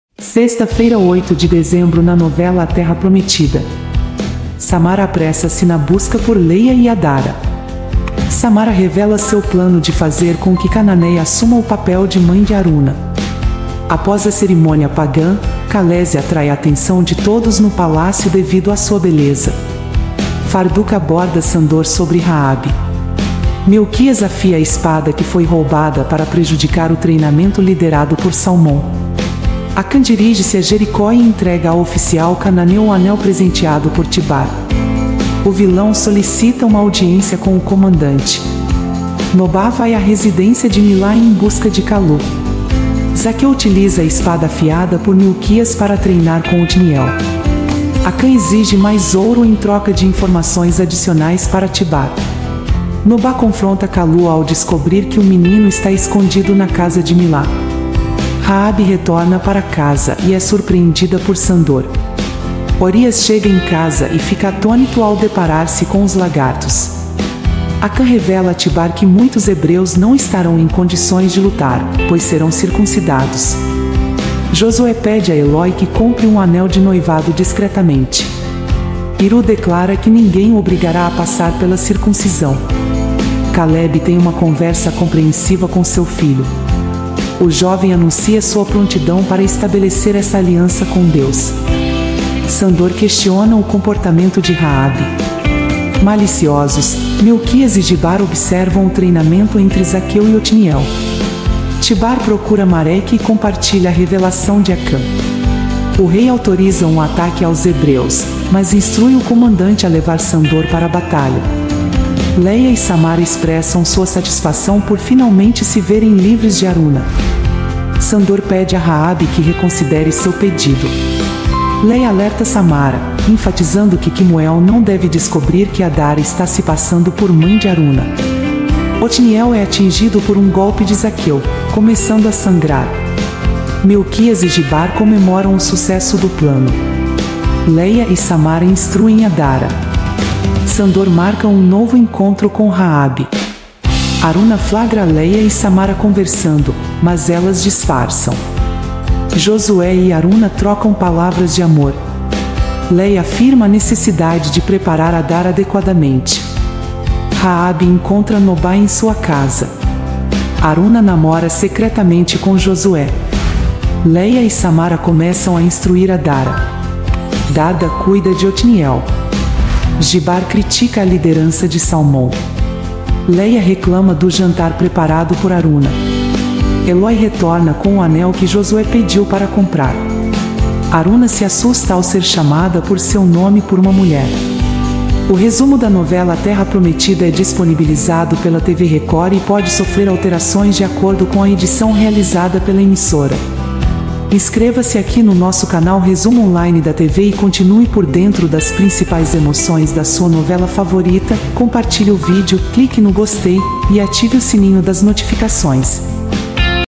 😍A TERRA PROMETIDA Capítulo de SEXTA-FEIRA 08.12.23 Resumo de hoje da Novela A TERRA PROMETIDA
🔴Seja membro deste canal e ganhe benefícios:
🔴A TERRA PROMETIDA Capítulo de SEXTA-FEIRA 08.12.23 Resumo de hoje da Novela A TERRA PROMETIDA |aterraprometida @resumoonlinedatv

🔴Vídeo:  resumo do capítulo 49 de hoje 08/12.

Veja agora o resumo da novela a terra prometida (08/12 de 2023) e saiba o que vai acontecer nos próximos capítulos.

Samara apressa-se na busca por Leia e Adara. SAMARA revela seu plano de fazer com que Cananéia assuma o papel de mãe de haruna. Após a cerimônia pagã, Kalési atrai a atenção de todos no palácio devido à sua beleza. Farduk aborda Sandor sobre Raabe. Melquias afia a espada que foi roubada para prejudicar o treinamento liderado por Salmon. Acã dirige-se a Jericó e entrega ao oficial cananeu o anel presenteado por Tibar. O vilão solicita uma audiência com o comandante. Nobá vai à residência de Miláh em busca de Kalu. Zaqueu utiliza a espada afiada por Melquias para treinar com Otniel. Acã exige mais ouro em troca de informações adicionais para Tibar. Nobá confronta Kalu ao descobrir que o menino está escondido na casa de Miláh. Raabe retorna para casa e é surpreendida por Sandor. Orias chega em casa e fica atônito ao deparar-se com os Lagartos. Acã revela a Tibar que muitos hebreus não estarão em condições de lutar, pois serão circuncidados. Josué pede a Elói que compre um anel de noivado discretamente. Iru declara que ninguém o obrigará a passar pela circuncisão. Calebe tem uma conversa compreensiva com seu filho.